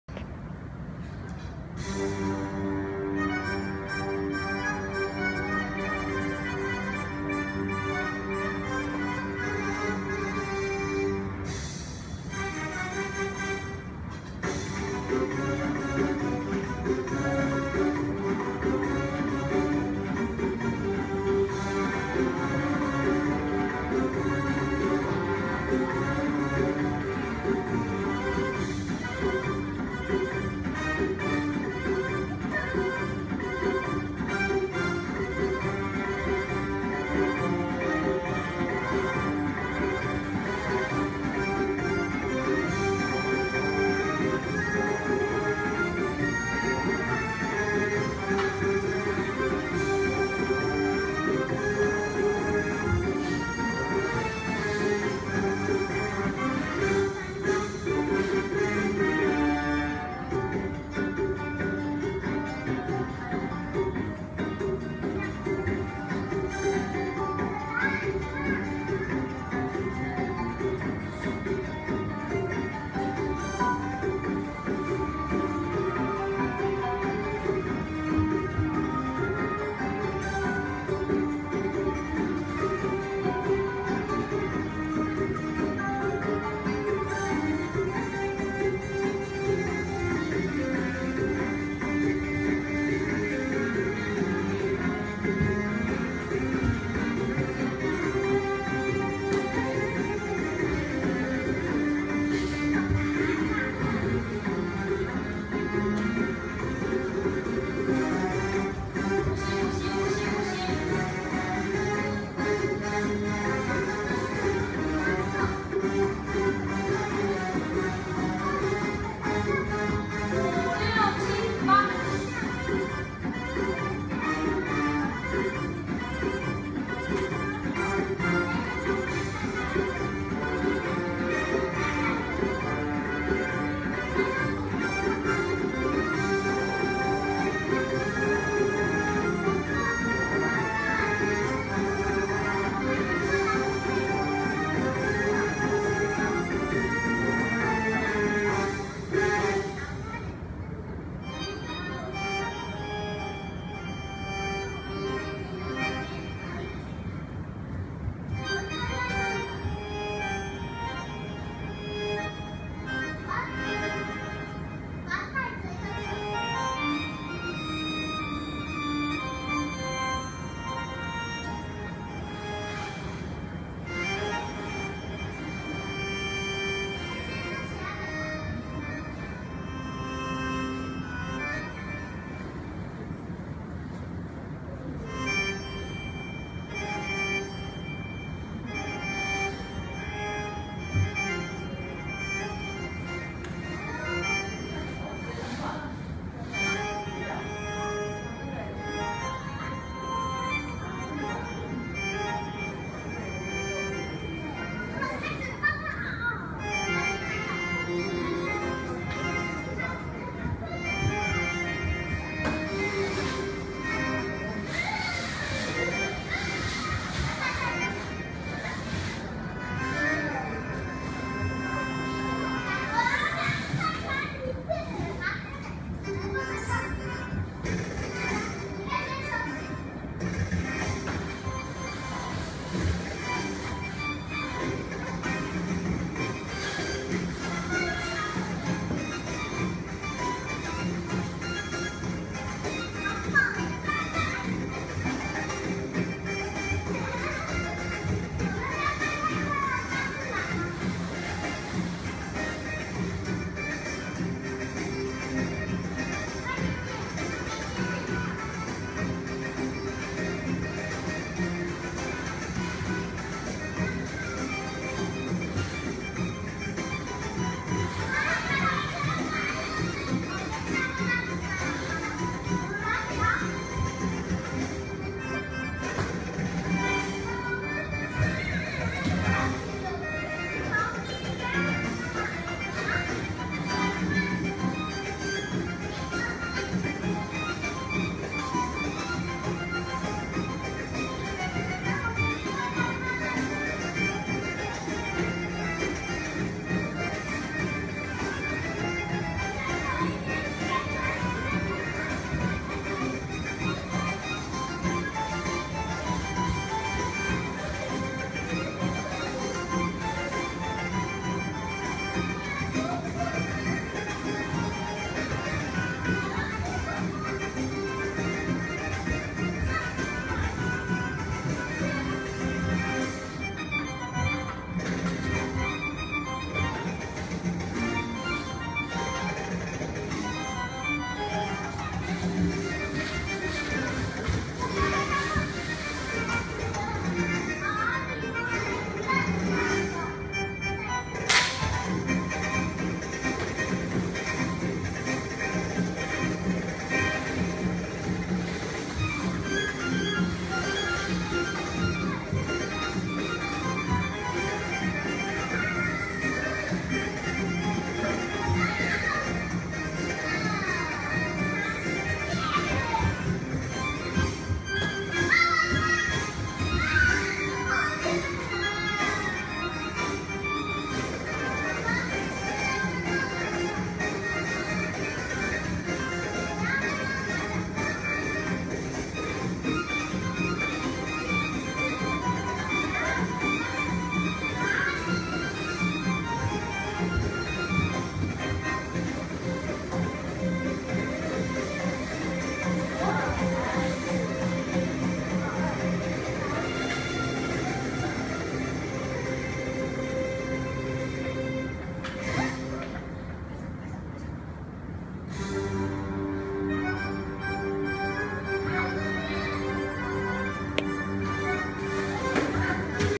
C's Bomb Bomb 上台前最後一次練習
為了Nour & Yasser首次來台灣的Gala show
七彩棒棒們拚了!